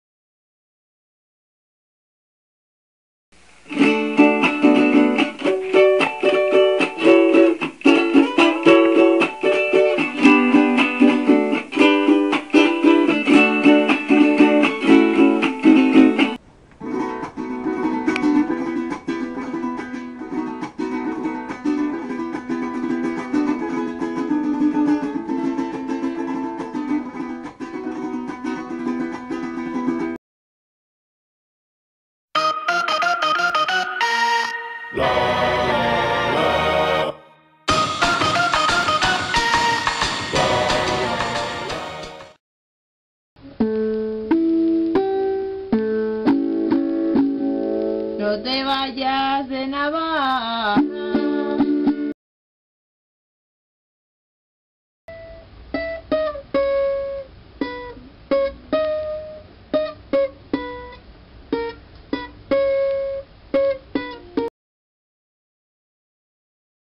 Tocando el cuatro
Pues eso, que lo tenía ahí en su funda y esta semana he empezado a investigar cómo se toca aunque de momento no suene como debería.

Ah, me falta cantante para la jota pero por aquí no me han querido colaborar. En el próximo video americano ya podíais estiraros y entonar algún blues, que estais en América!